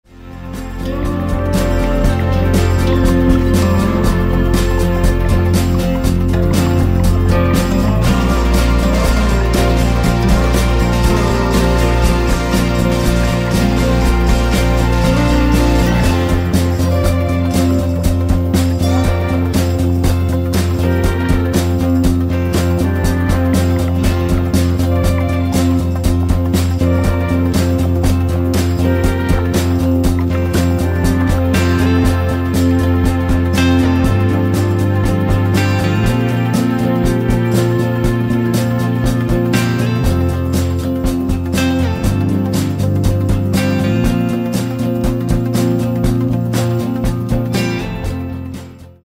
Innenaufnahme Wohnwagen Hymer Eriba Feeling 442 Modelljahr 2024
Fahrzeugbegehung des Hymer Eriba Feeling 442 Wohnwagens Modelljahr 2024.